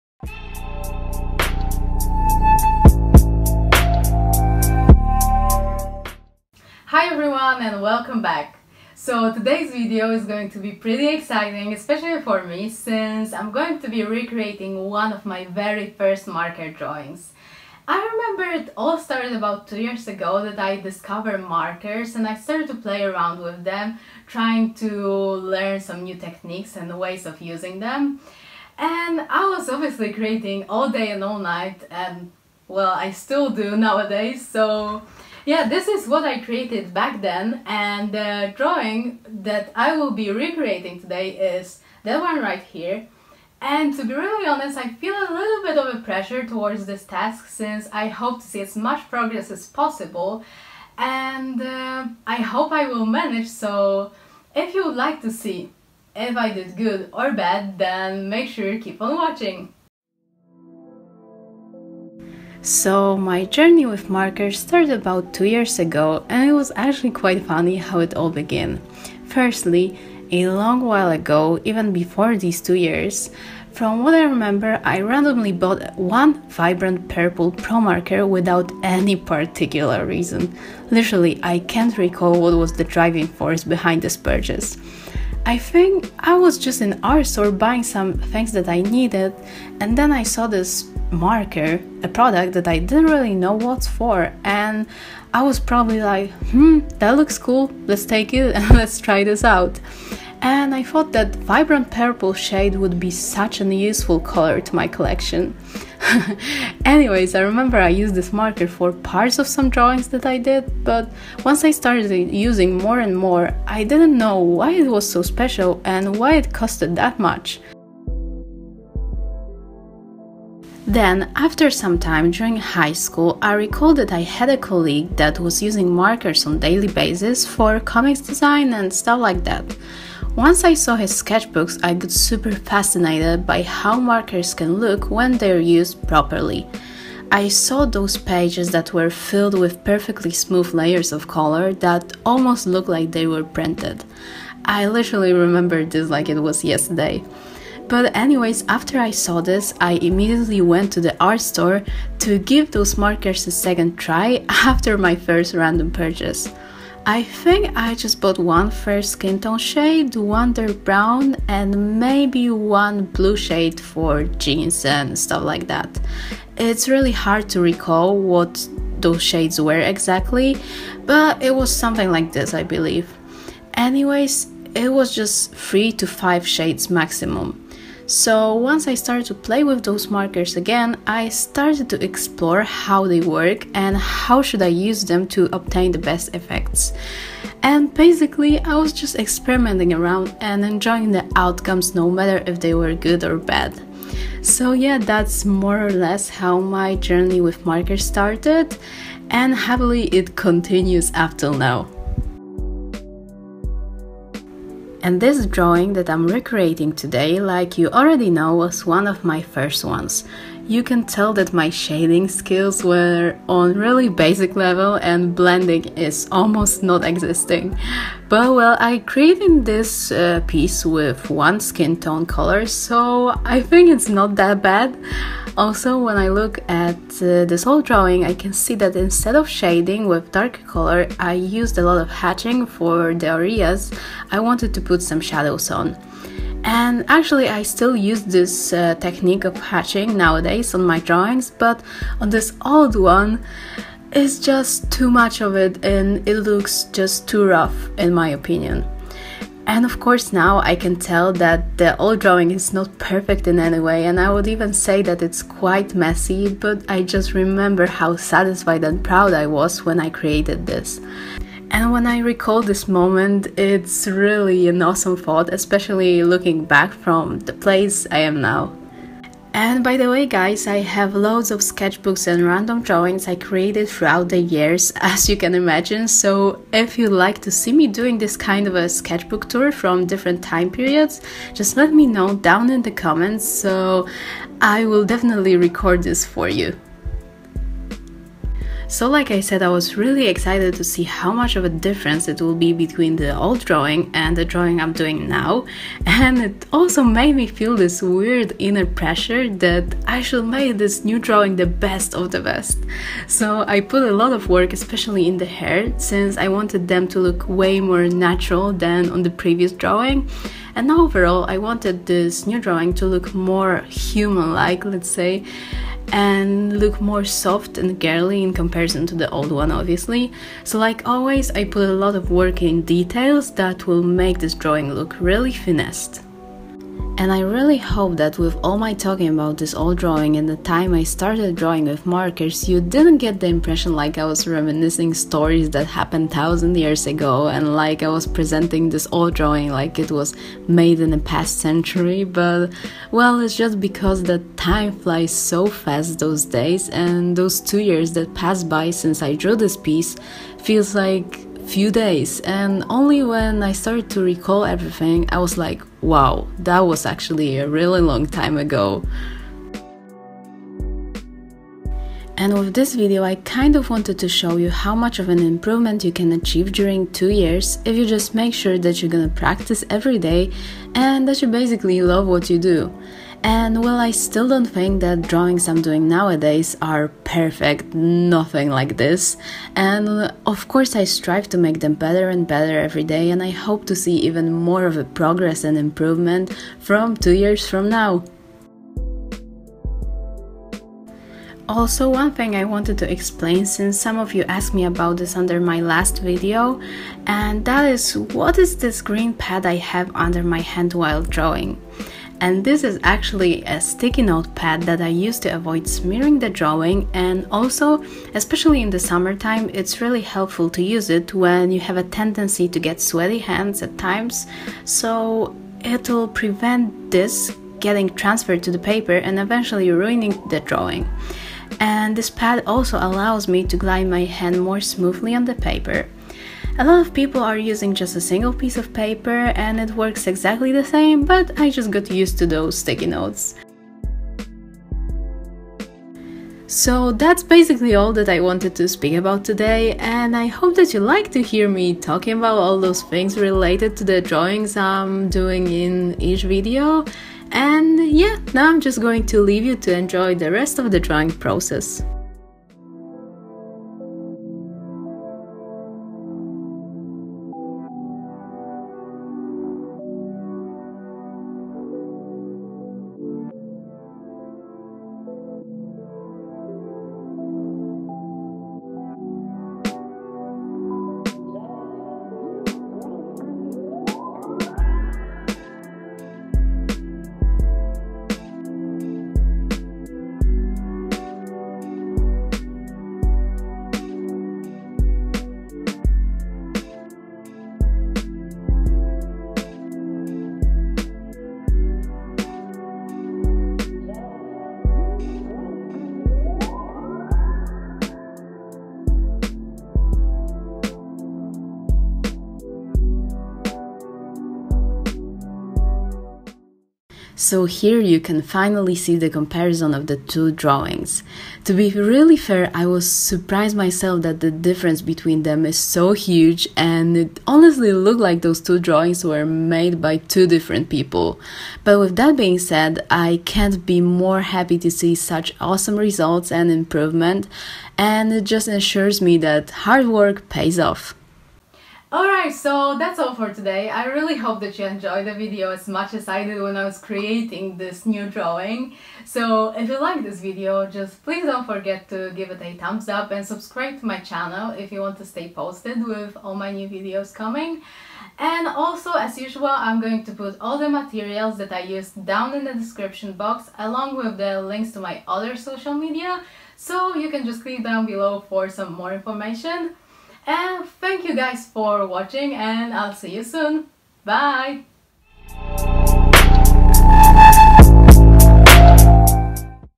DRAW THIS AGAIN: Recreating a Drawing from 2015
▷ MATERIALS I USED IN THIS VIDEO:

PAPER:
- Canson Marker Pad XL

Pencils:
- Prismacolor Col-Erase Lavender
- Automatic Pencil 0.5 3B
- Automatic Pencil 0.5 HB

Fine Liners:
- Micron Fine Liner 0.05
- Micron Fine Liner 0.1
- Uni Pin Fine Liner 0.05

Markers:

PROMARKERS:
Dusky Pink, Almond, Umber, Cool Grey 1, Cool Grey 2, Cool Grey 4, Denim Blue, Midnight Blue

COPIC MARKERS:
E000, E41, E42, B95, B99, E44, RV93

Colored Pencils:
- Faber-Castell Polychromos